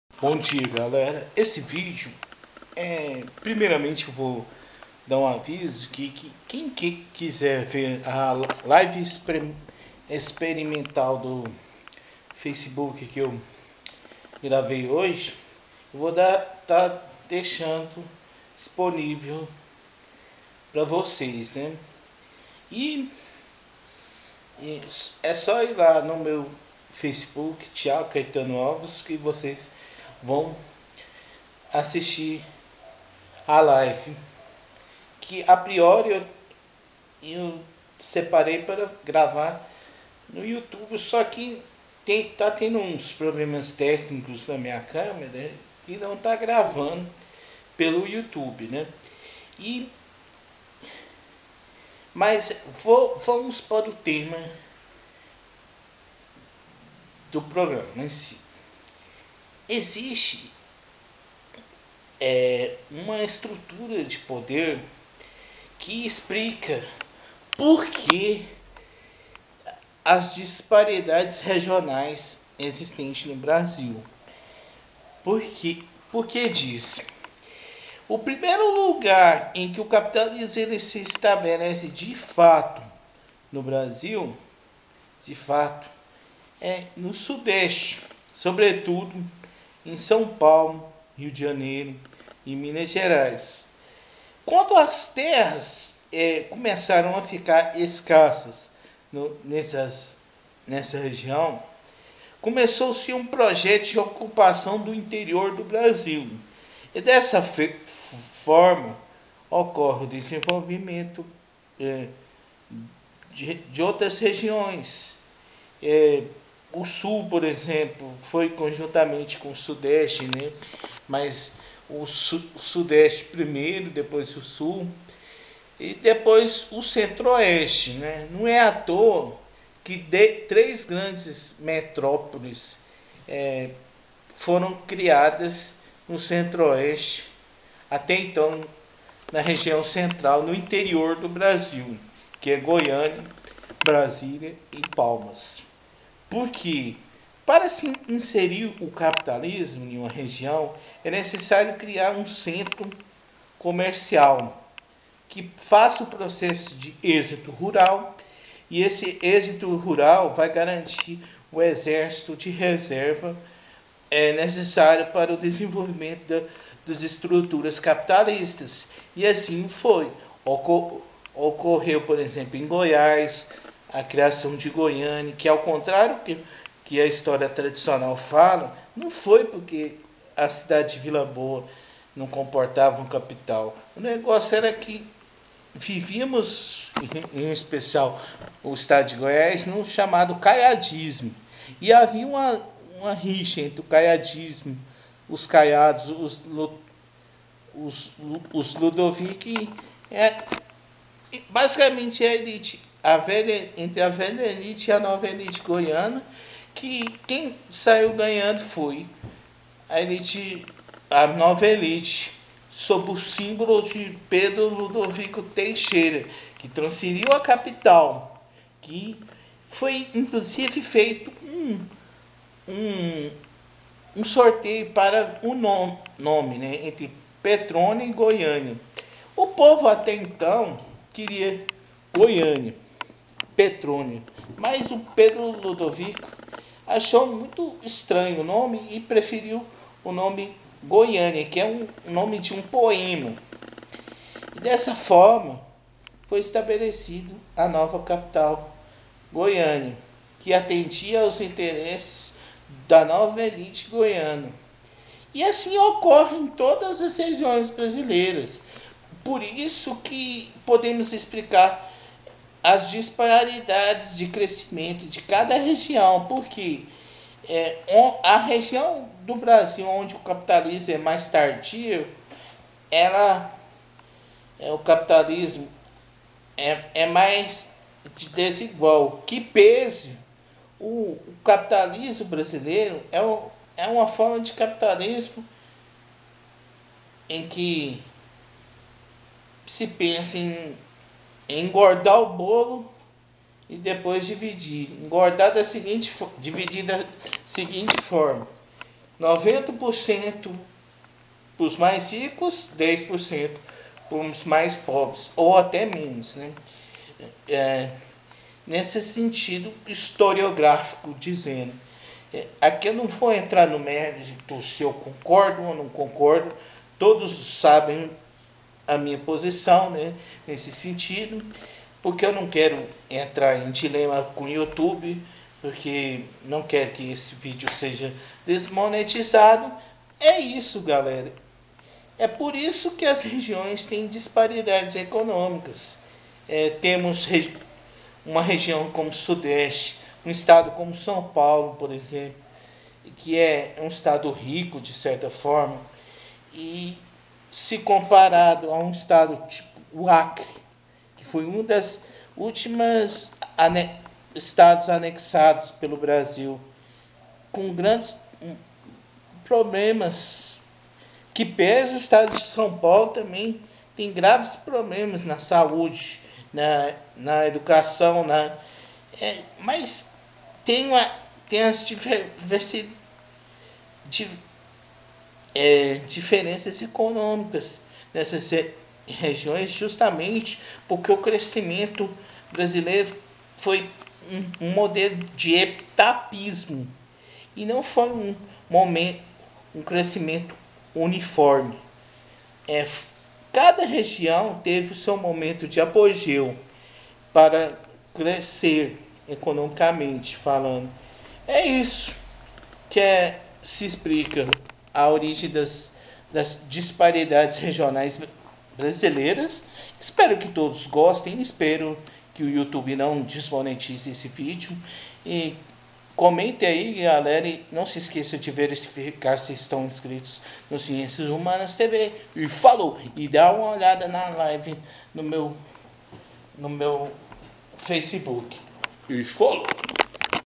Quais as causas das desigualdades regionais?
O propósito do vídeo é explicar a falta de planejamento econômico que levou a alta disparidade econômicas entre as regiões.
Pesquisas
Dailymotion